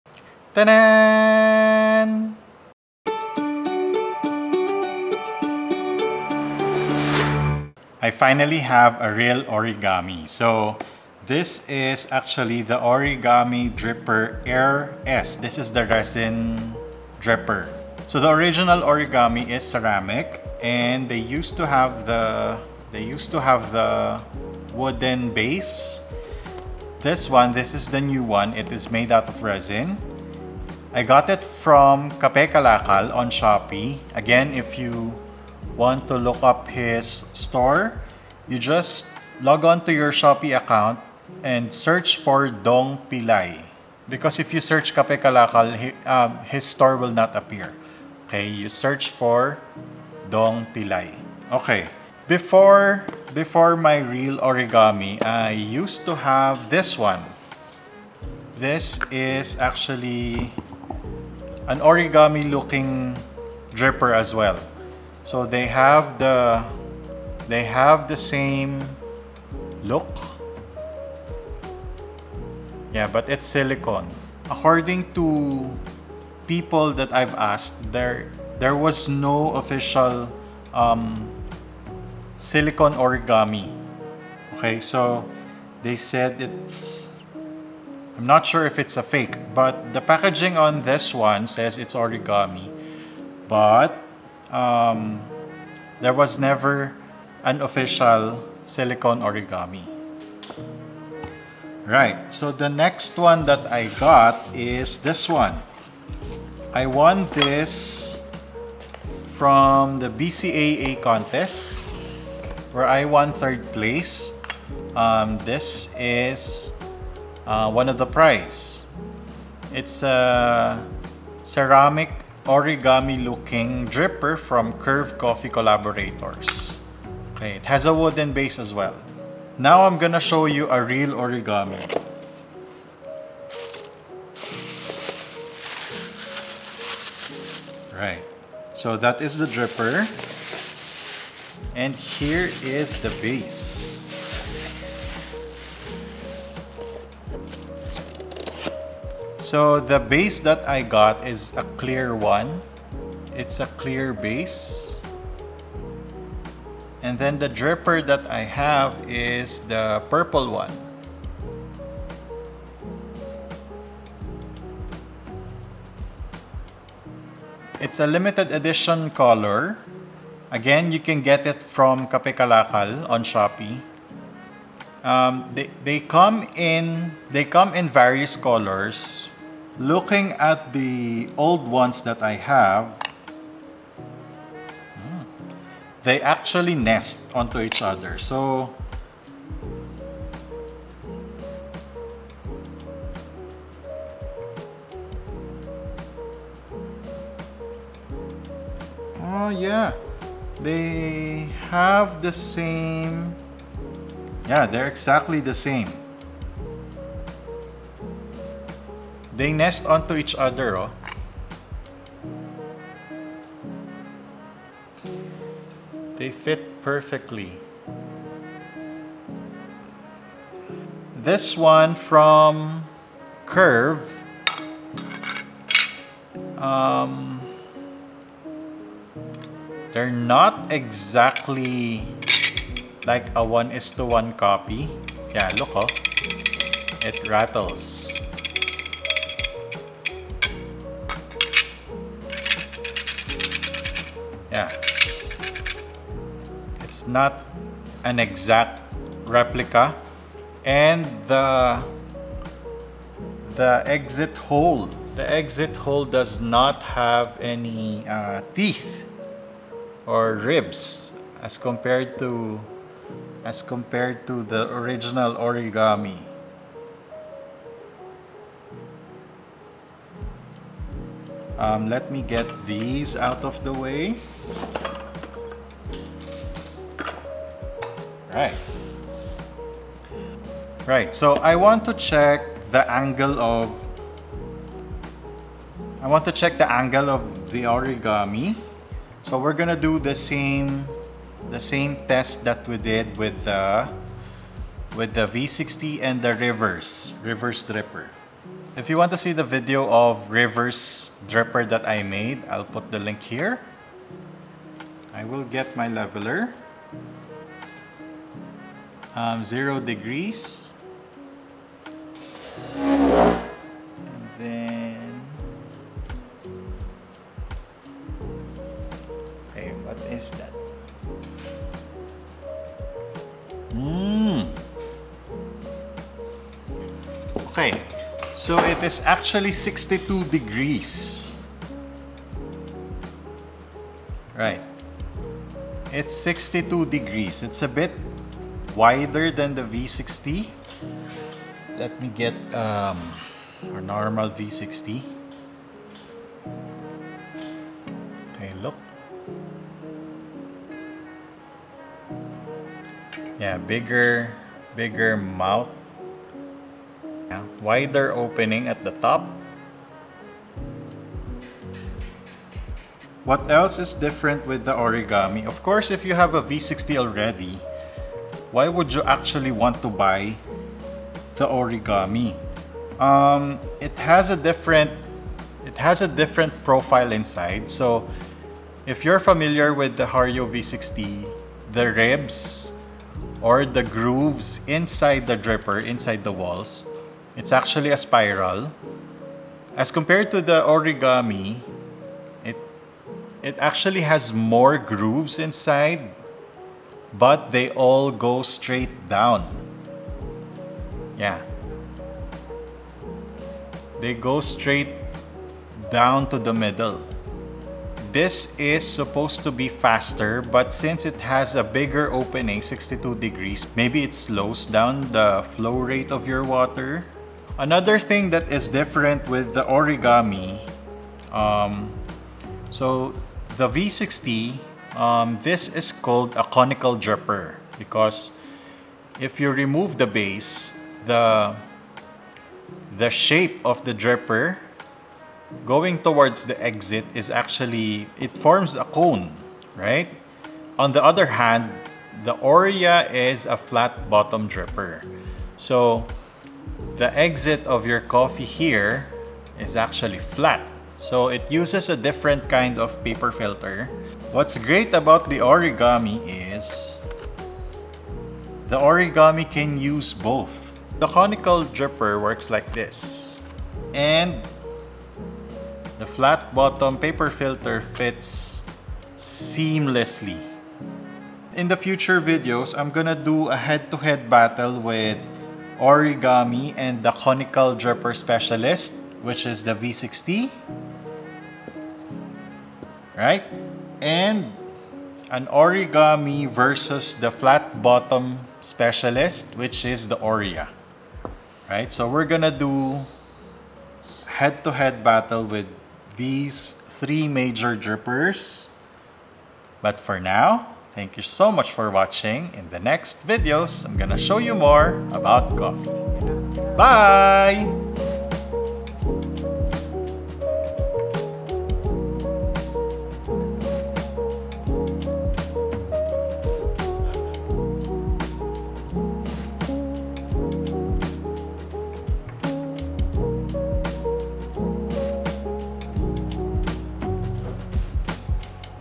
Origami Dripper
I finally have an Origami Dripper! Im so excited to learn its advantages from other drippers. People have been telling me to get this a long long time ago now and I dont understand why. Join me unbox my Origami inpurple of course!